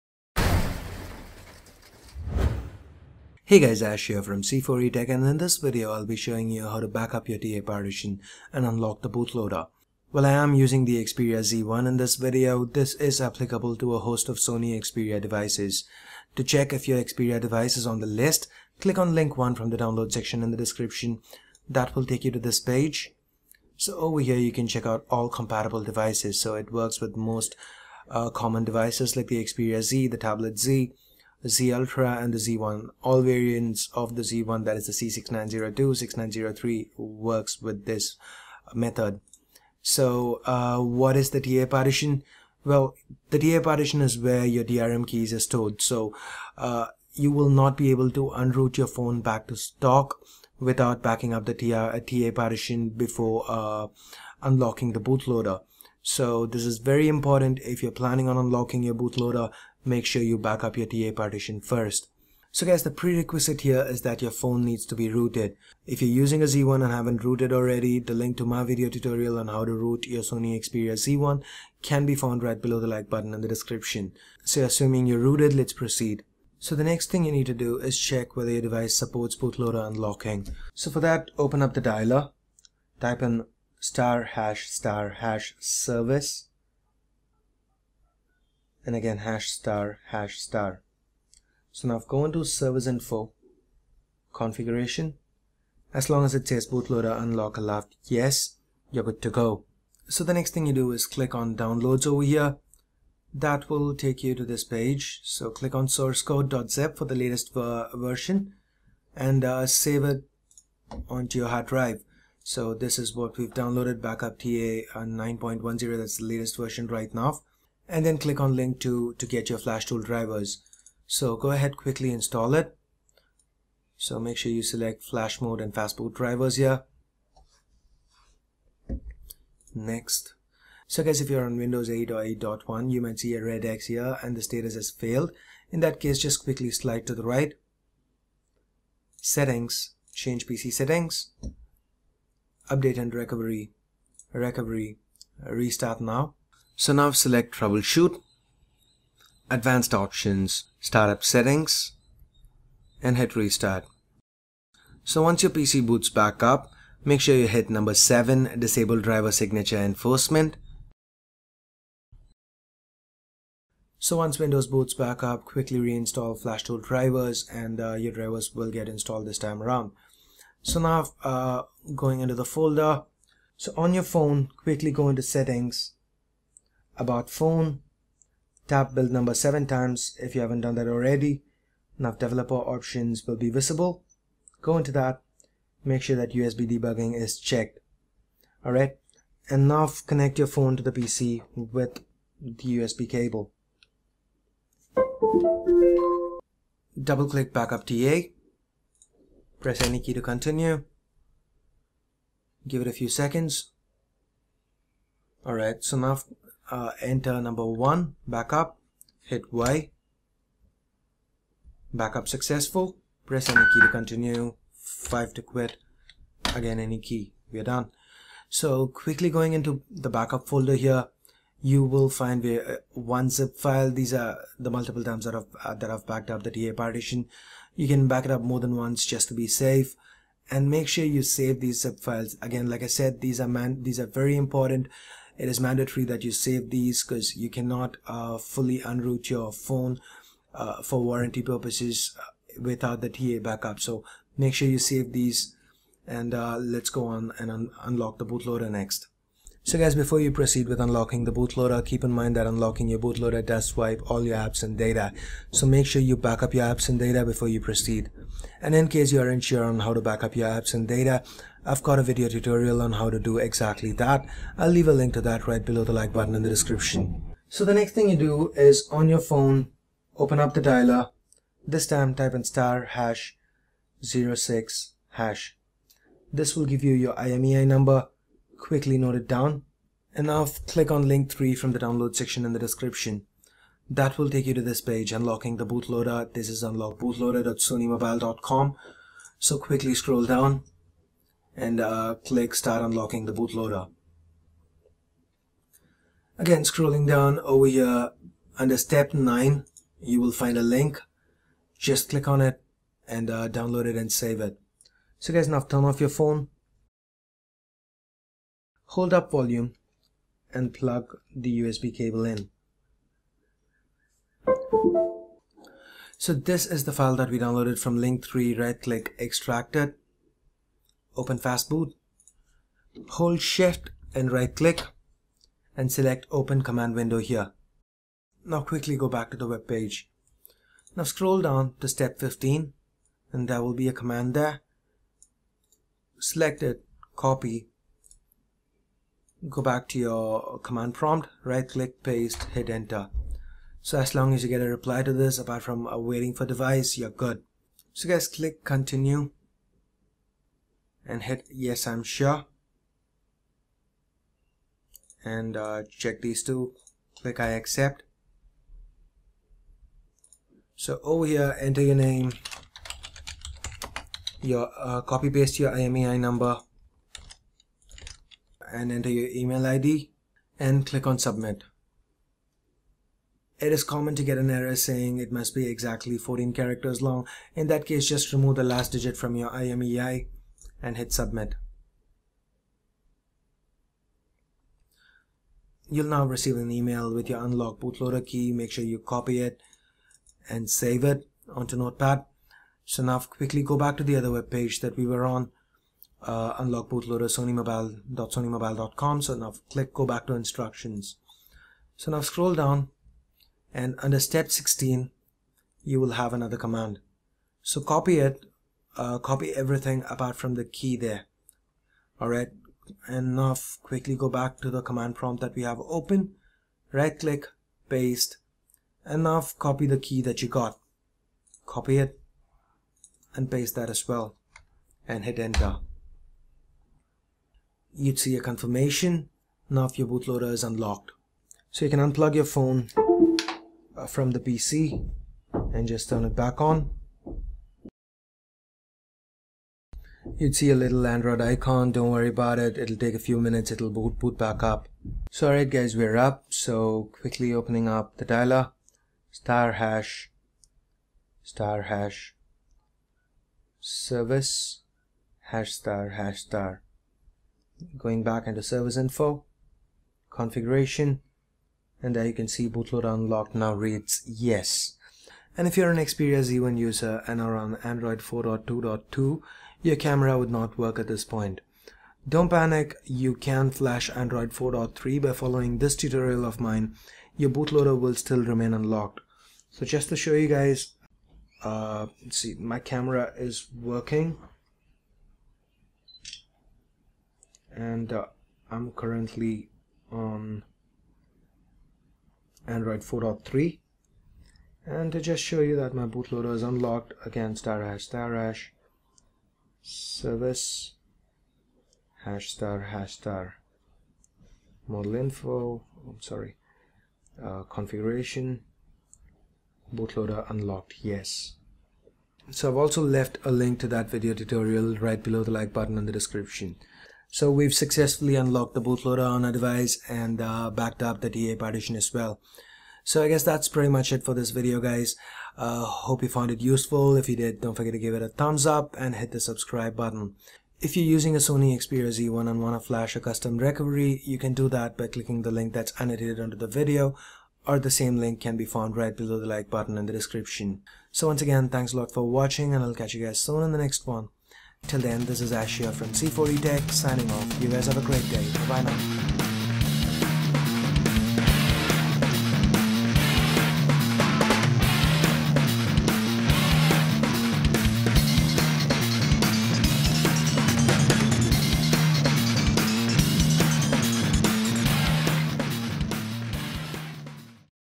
How to Unlock the Bootloader and Backup the TA Partition on Xperia Z1/Z/Z Ultra/M & more...
This is my video tutorial on how to unlock your bootloader.

This is applicable for a lot of Sony Xperia Devices including the Z1,Z,Z Ultra,M,Tipo,Sola and many more...

Download Links

To buy from the Amazon International Store

To buy from the Amazon India Store

Credits

DooMLoRD for his work on everything Sony Xperia :)

DevShaft for Backup TA